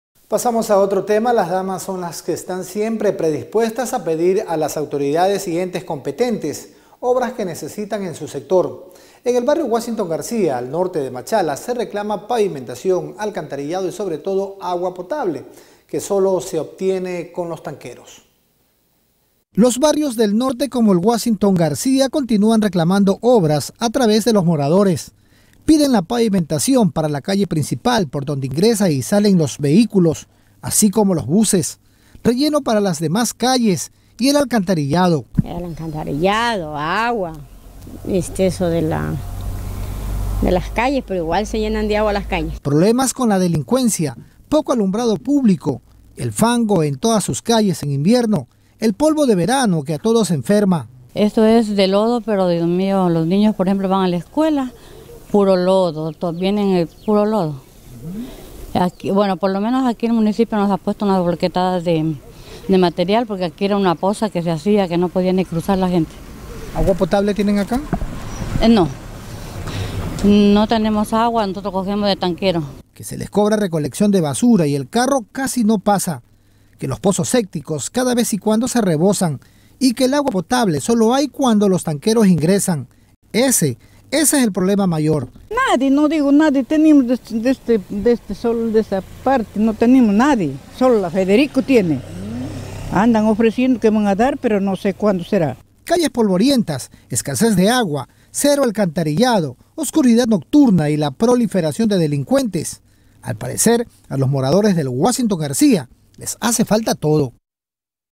Barrio washington garcía pide pavimentación
Las damas son las que están siempre predispuestas a pedir a las autoridades y entes competentes, obras que necesitan en su sector. En el barrio Washington García, al norte de Machala, se reclama pavimentación, alcantarillado y sobre todo agua potable, que solo se obtiene de los tanqueros.